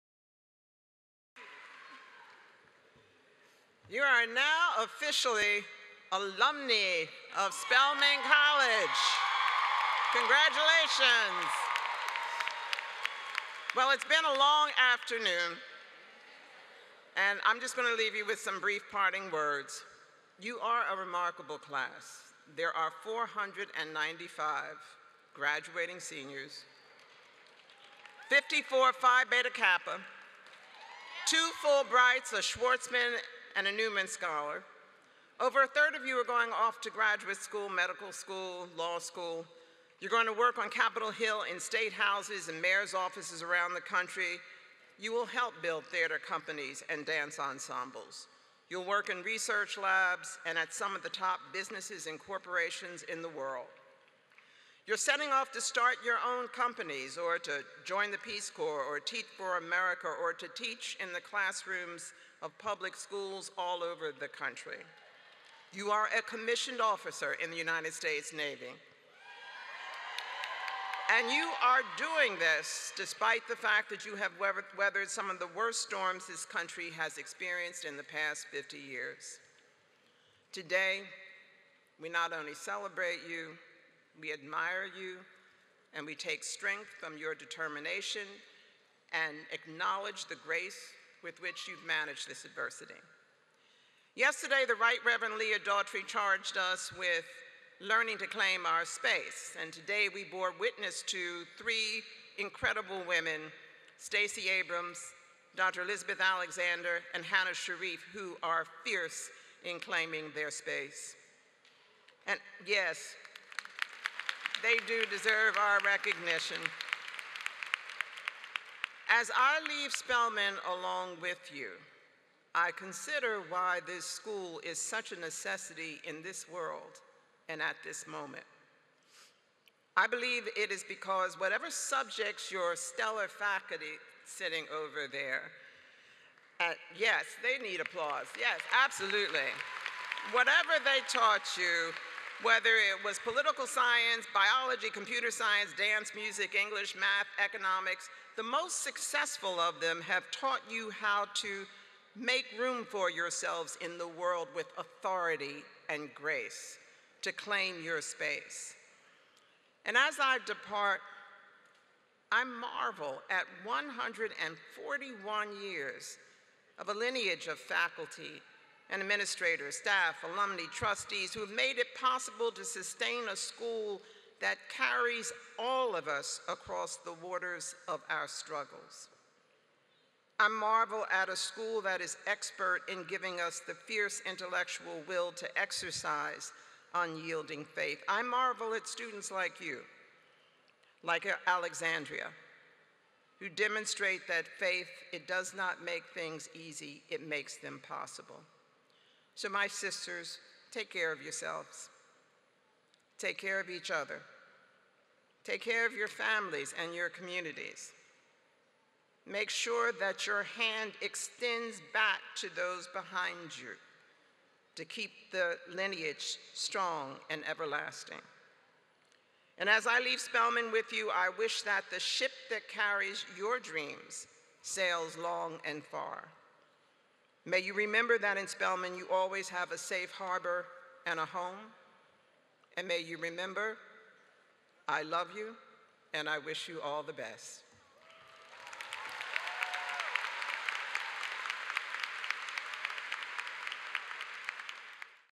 Spelman Commencement 2022 - Dr. Mary Schmidt Campbell, 10th College President, Charges Graduates
Relive Spelman’s 135th Commencement ceremony that took place on Sunday, May 15, 2022. Politician, lawyer and author, Stacey Y. Abrams, C'95, delivered the Commencement address, and Dr. Elizabeth Alexander, president of The Mellon Foundation, decorated poet, educator, memoirist, scholar, and cultural advocate received an Honorary Degree. The college celebrated five valedictorians, Nailah Barnes, Ashli Chicks, McKenzie Clarke, Taylor Dews, and Asja Woodard.

Spelman College does not own the rights to all the music featured in this broadcast.